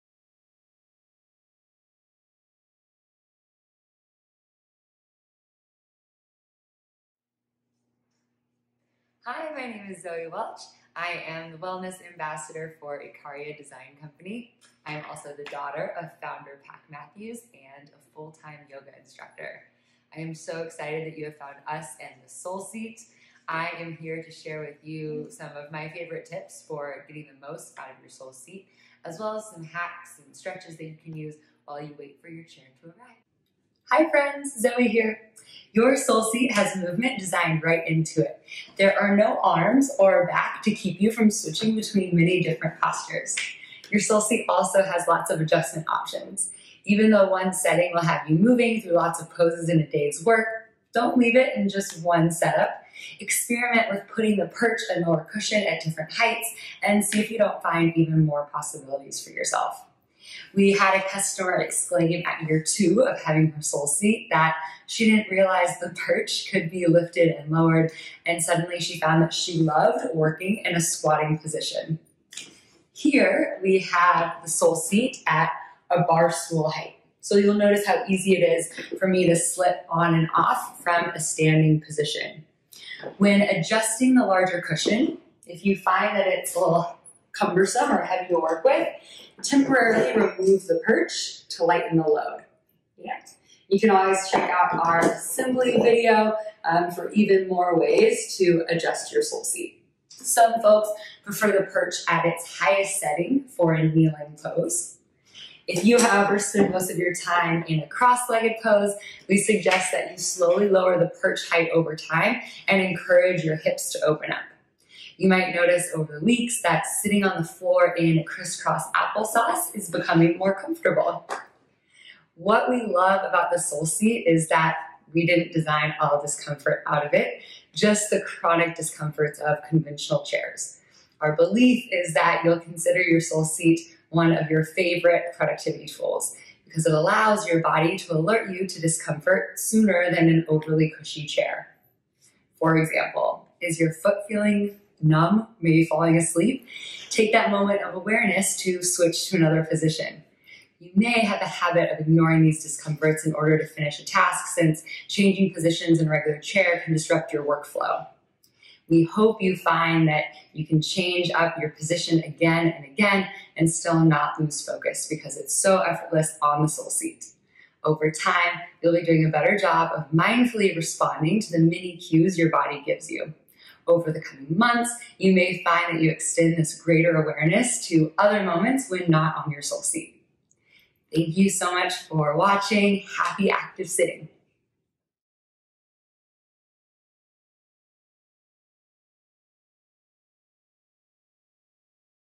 Your Soul Seat: Don't just sit there!
In this video, Zoë explains the various active sitting positions and height adjustments available to you on your new Soul Seat.